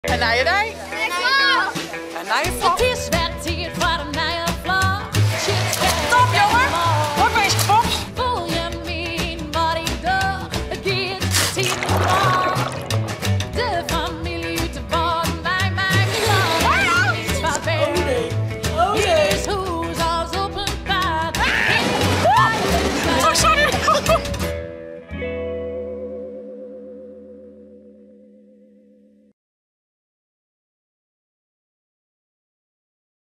Dit Binne Wy Ek! // Tjits Trailer
Ik bin Tjits en alle dagen upload ik in Fryske floch oer myn libben tegearre mei Jelke, Sake en Yldau yn Broeksterwâld! Dit Binne Wy Ek! // Tjits Trailer